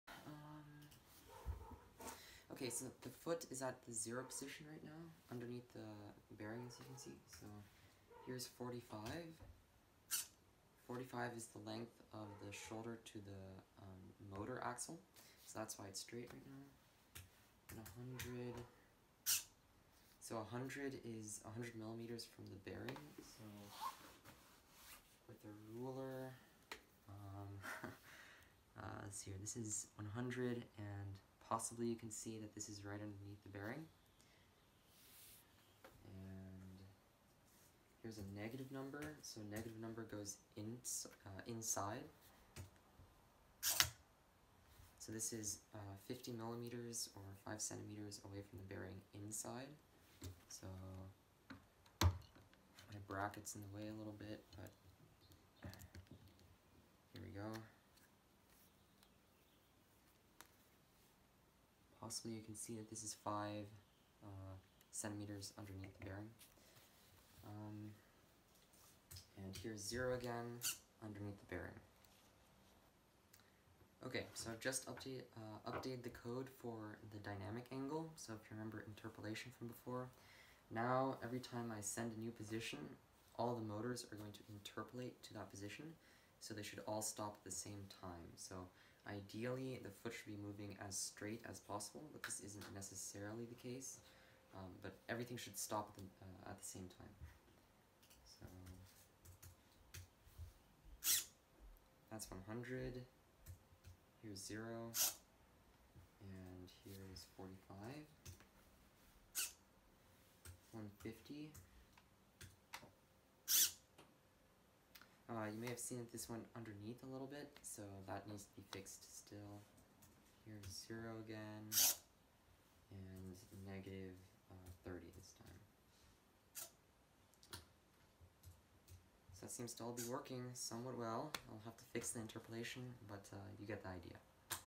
Quadruped Y-axis movement demo
Just a demo of the y-axis movement for my quadruped leg.

Please comment if you have questions!